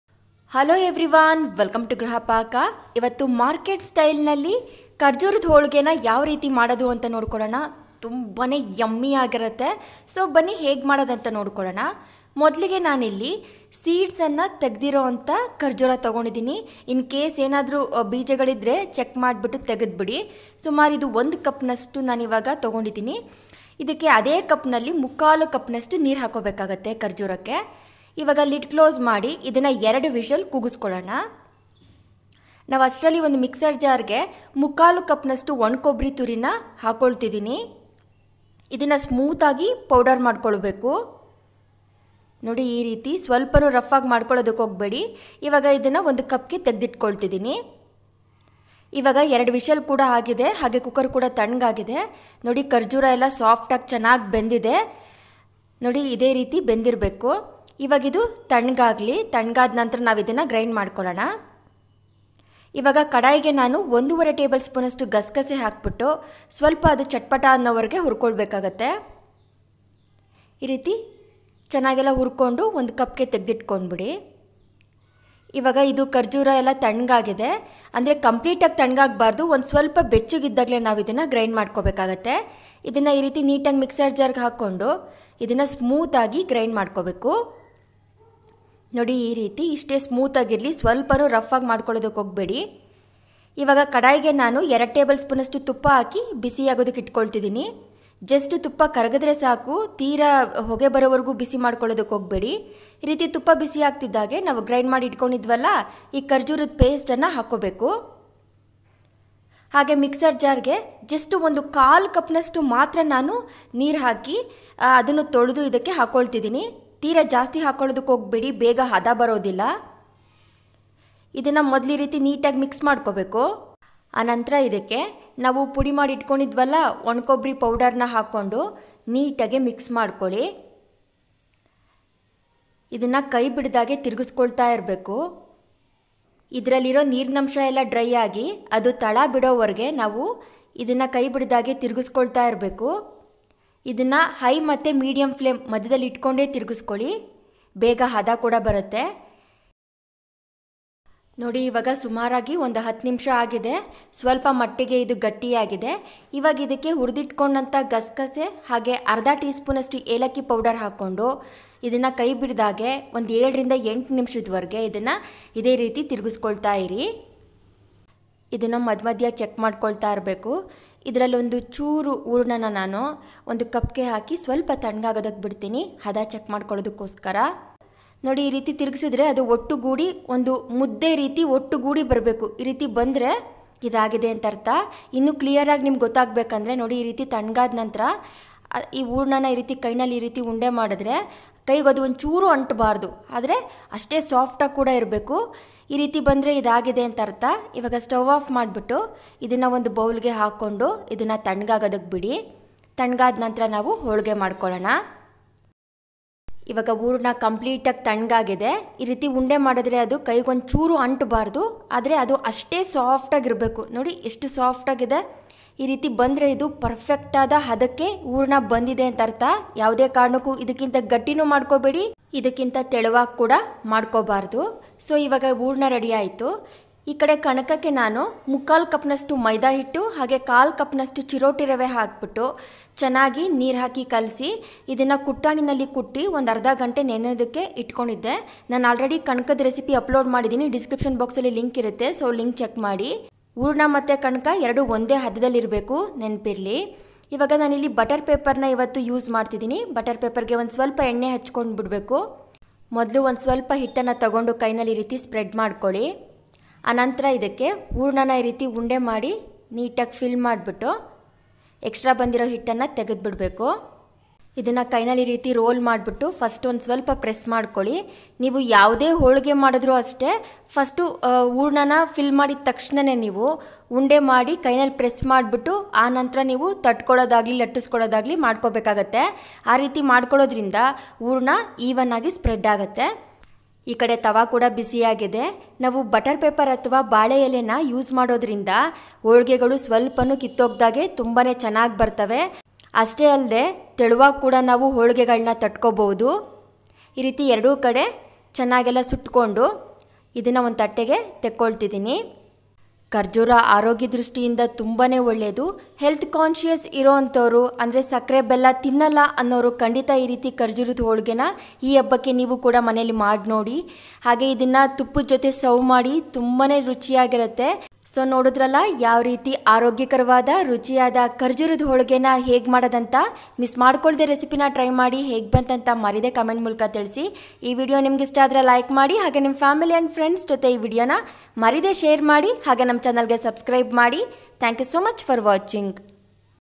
ನೀವೆಂದೂ ಮಾಡಿರದ ಮೃದುವಾದ ಖರ್ಜೂರದ ಹೋಳಿಗೆ ಮಾಡುವ ವಿಧಾನ| 100% Market style Dates holige
2 ರೀತಿಯಲ್ಲಿ ಮೃದುವಾದ ಹೋಳಿಗೆ ಹಿಟ್ಟು or ಕಣಕ ಮಾಡುವ ವಿಧಾನ| 2 types of Holige hittu